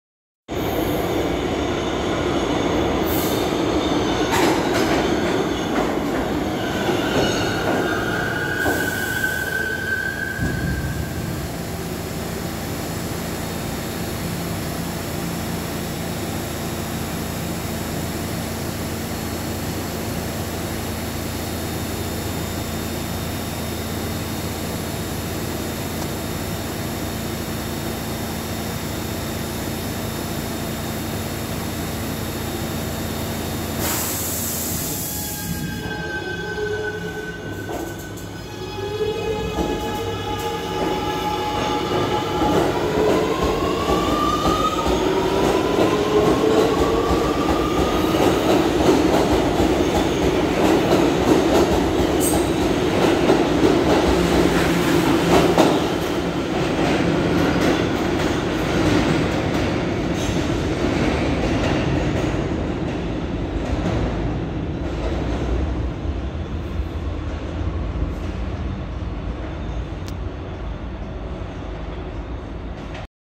R211T Set 4050-4054 Soft Shell at Utica Avenue
Filmed on the 14th July 2023
I'm expecting these to be ready by September or late August, can't wait to ride them!